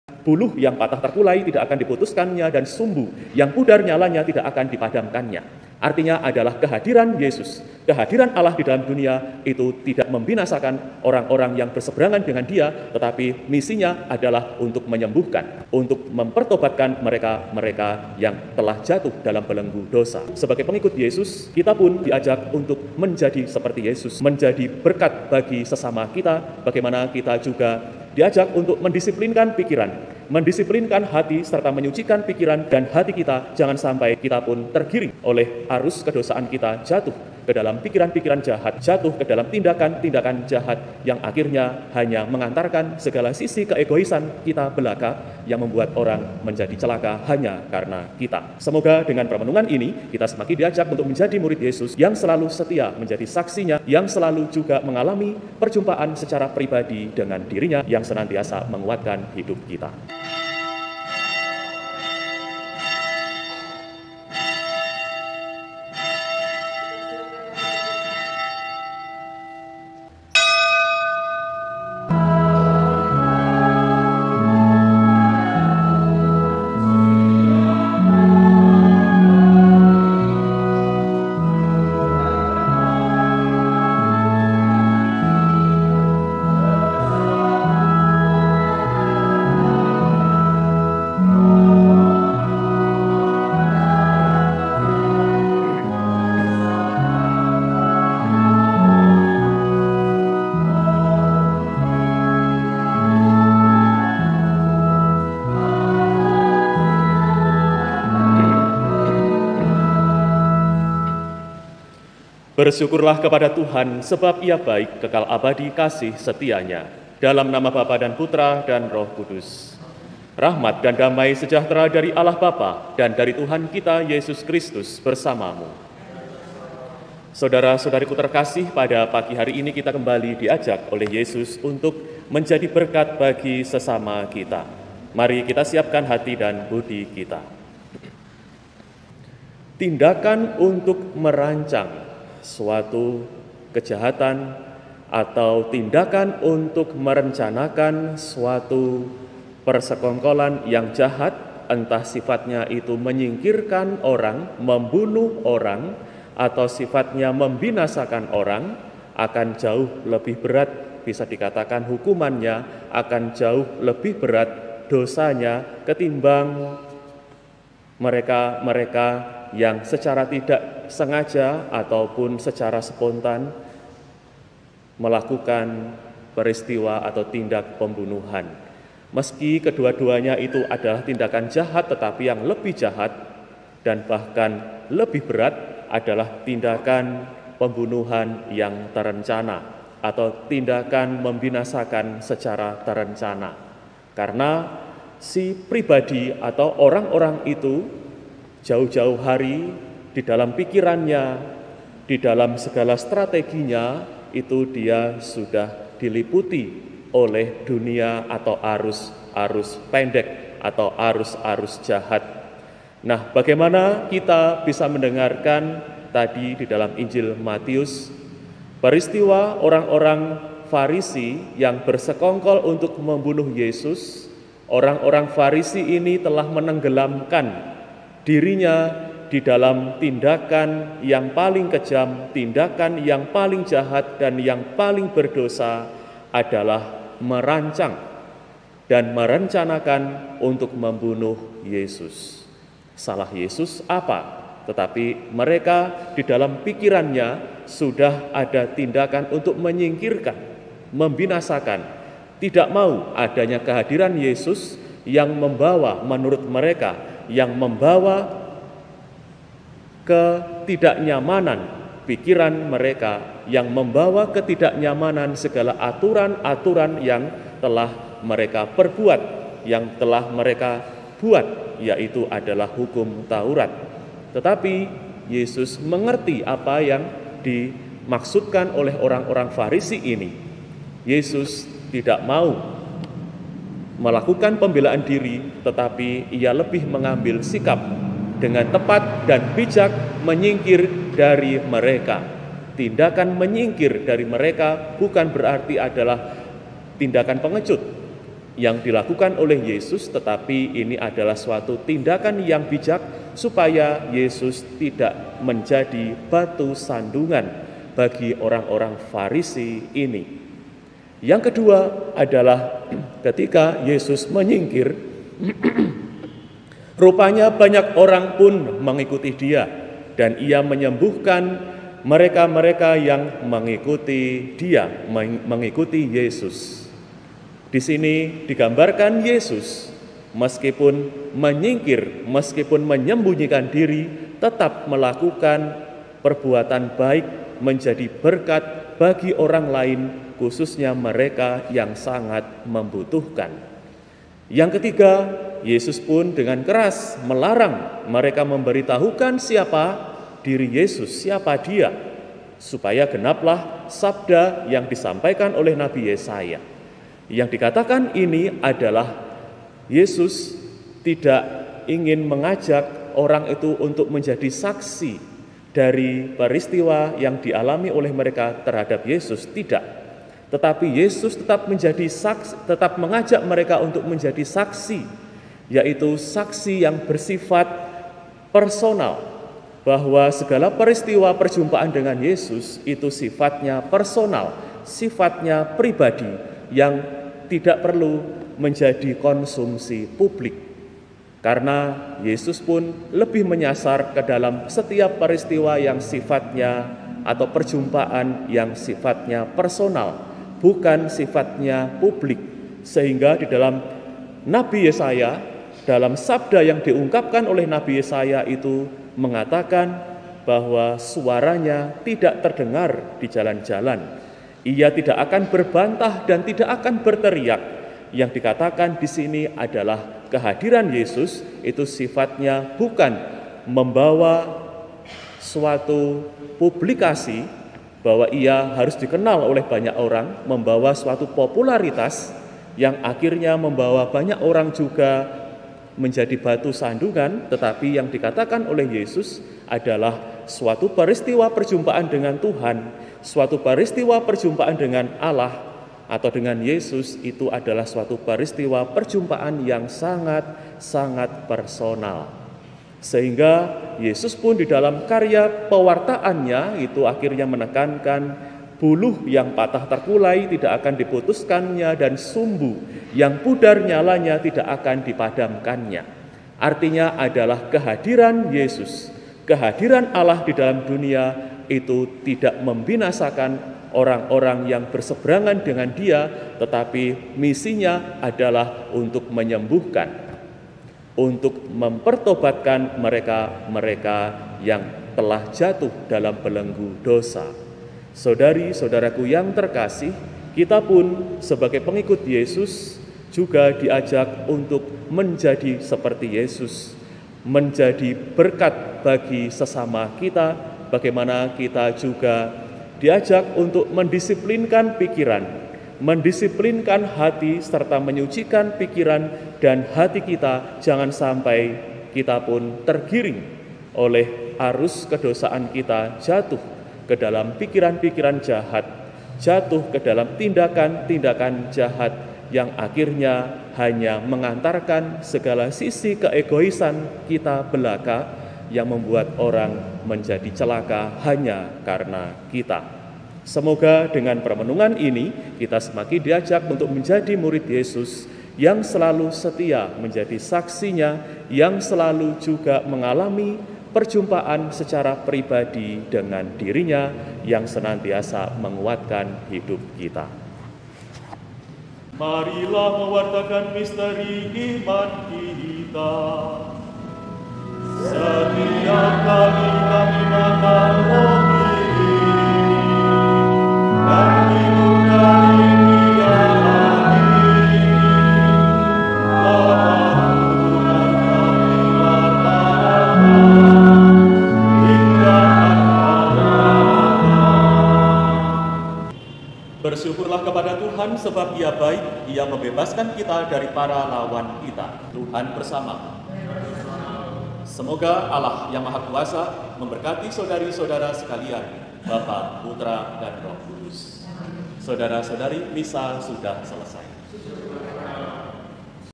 🀄16.07.2022🀄kita diutus menjadi berkat bagi sesama❗Homili RomoMichaelGratia@KomsosHKYTasikmalaya
Perayaan Ekaristi dipimpin oleh RD Michael Gratia Sekundana, Sabtu, 16 Juli 2022 Hari Biasa Pekan C/II

1:52 | Antifon Pembuka
2:07 | Pengantar
2:19 | Homili
9:12 | Anamnese
9:39 | Antifon Komuni

Mohon ijin & Terima kasih ya, dengan harapan yang hendak ambil bagian dalam Perayaan Ekaristi langsung ke chanel ‎@KomsosHKYTasikmalayaBerkah Dalem. 🙏